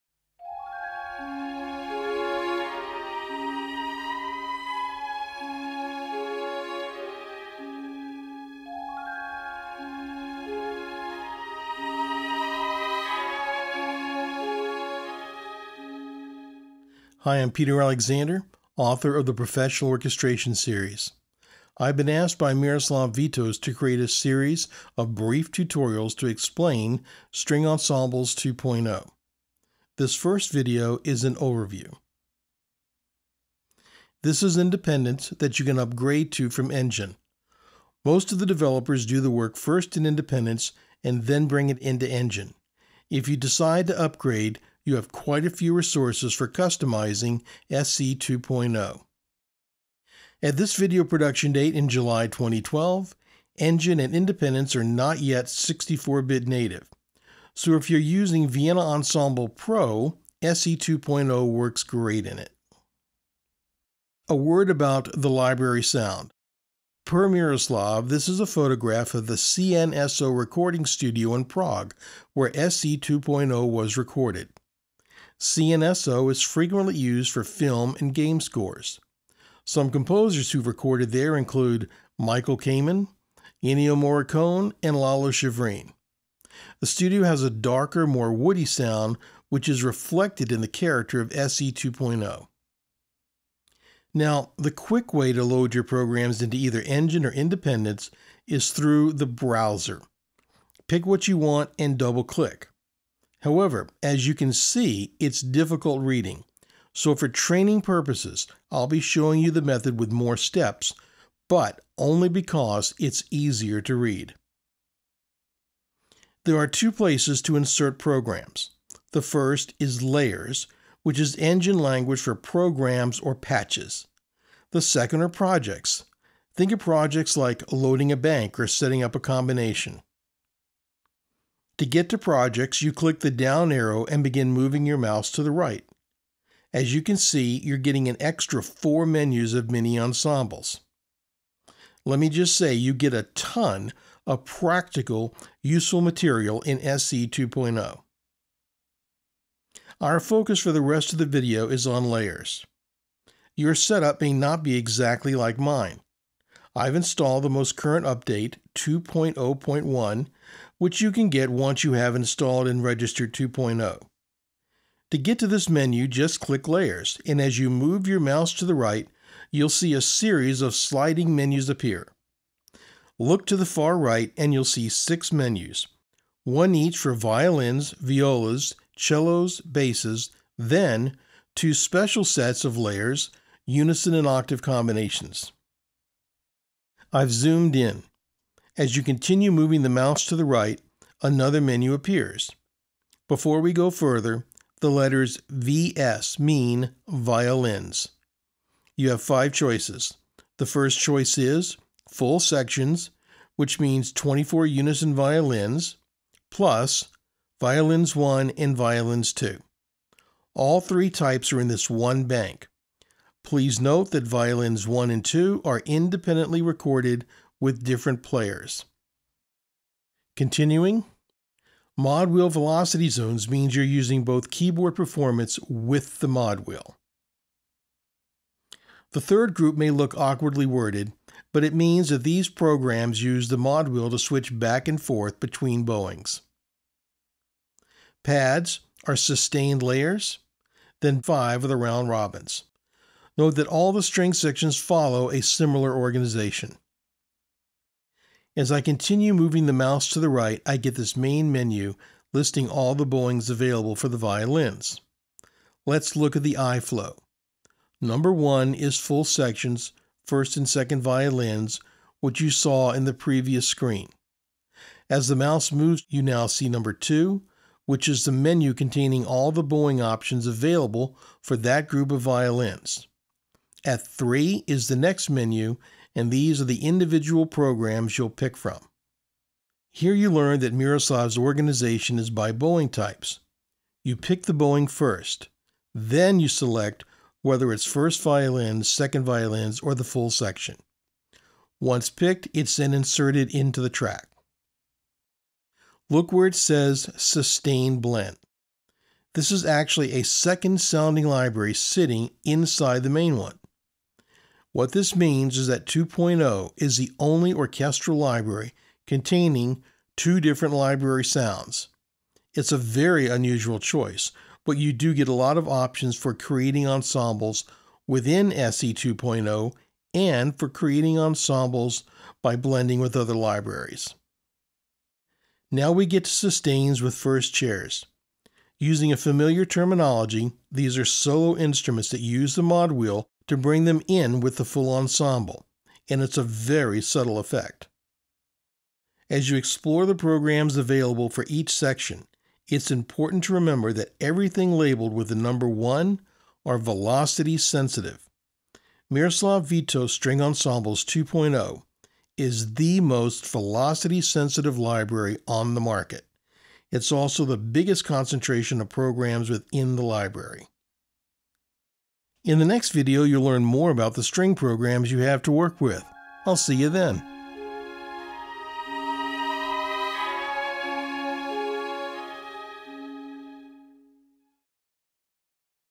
Miroslav Vitous String Ensembles 2.0 Training Video #1
Training Video #1: Overview. Opening music by Rohan Stevenson. This video explains library organization and how to load programs from Layers and Projects.